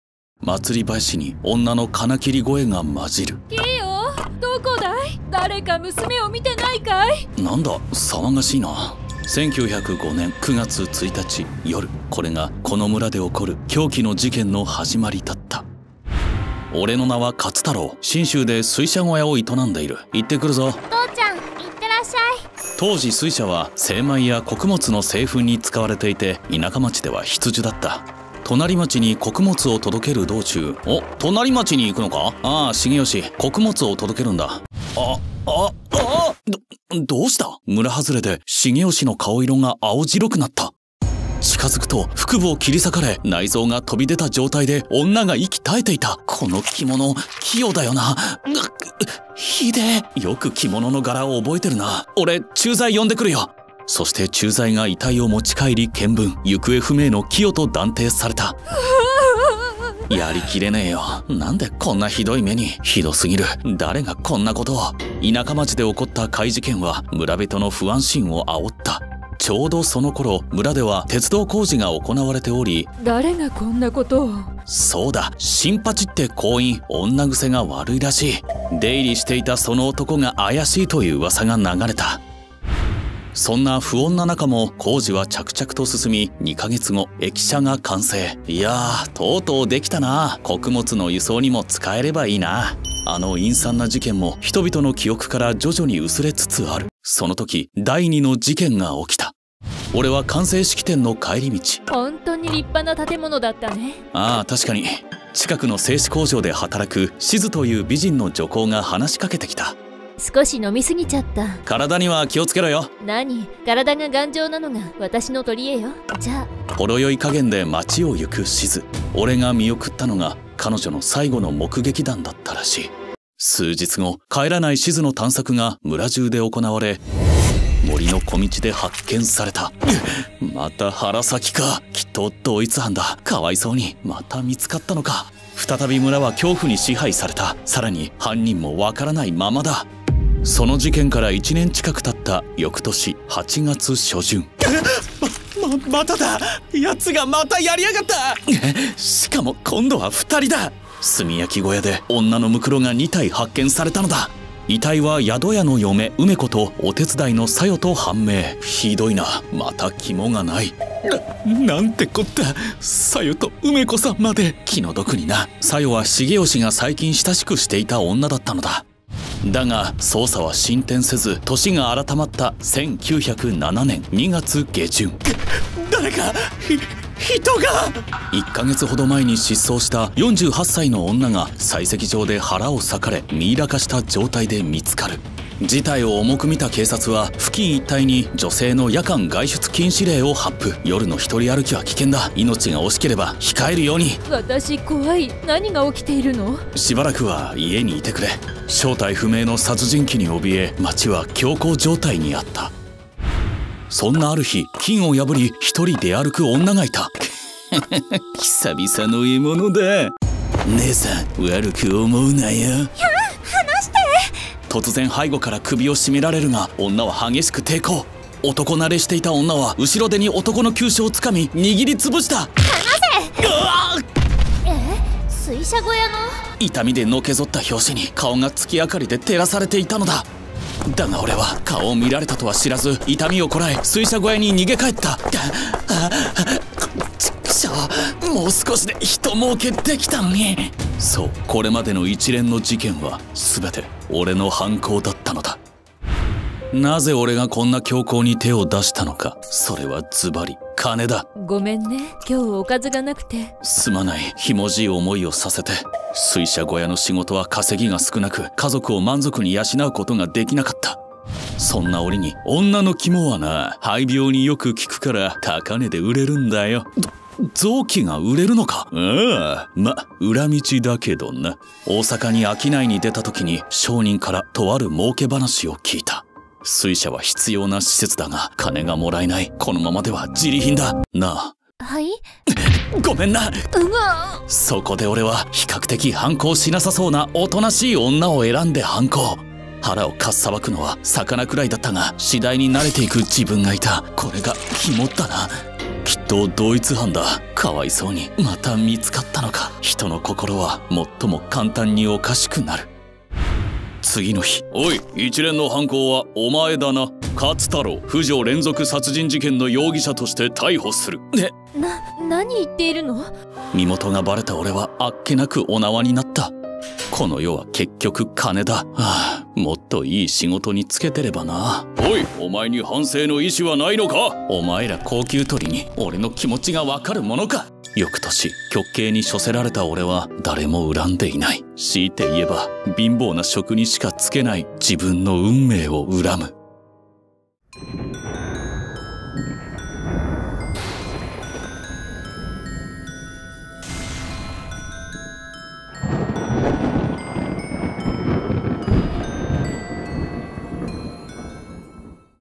【実話】女のアレ専門販売男。次々と奪って…瓶に詰めて売る。
漆黒アカデミアは、世の中のイカれた事情を漫画にし国内外の歴史を学んでいます。